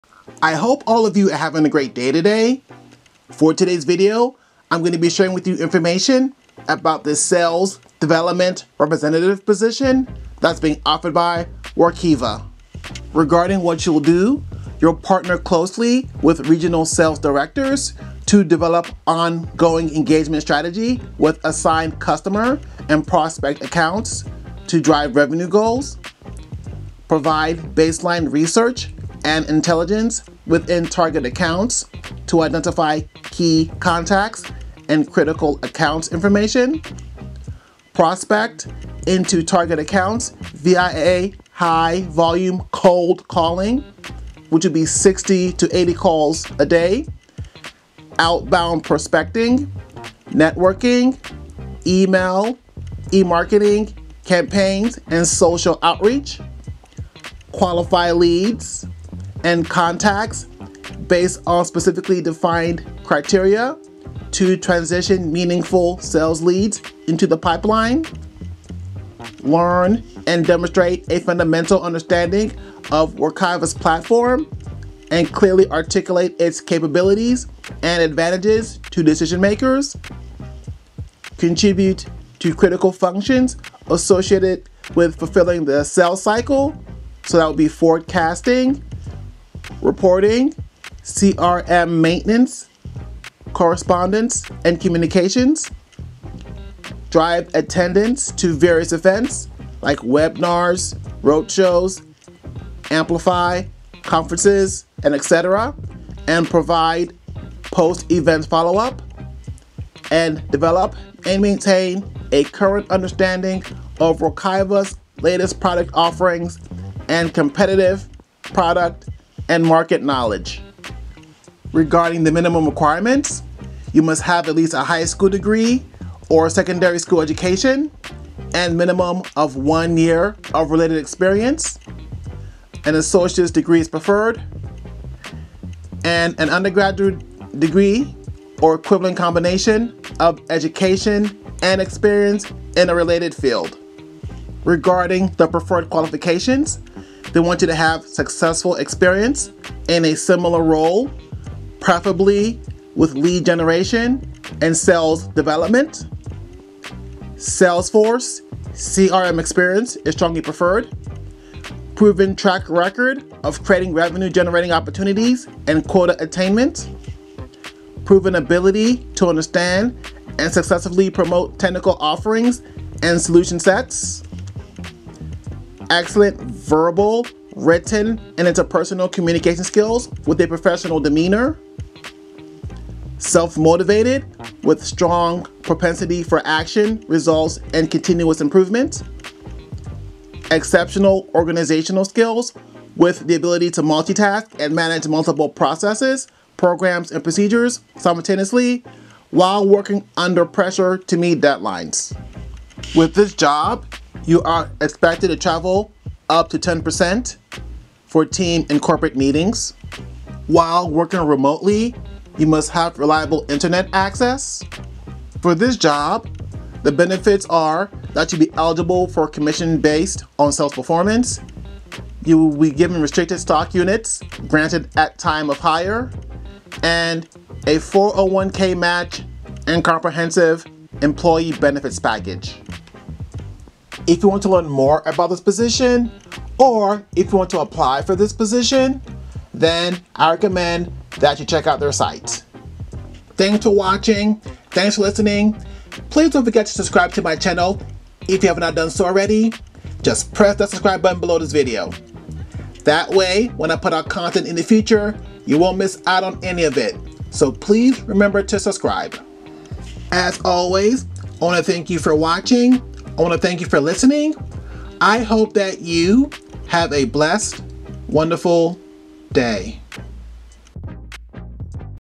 Work From Home Job/Great Work Benefits Provided/Full time/$41,000 To $66,000 A Year
In this video I share a work from home job that provides great benefits. If you are looking for a work from home job that provides great benefits then I recommend that you watch this video.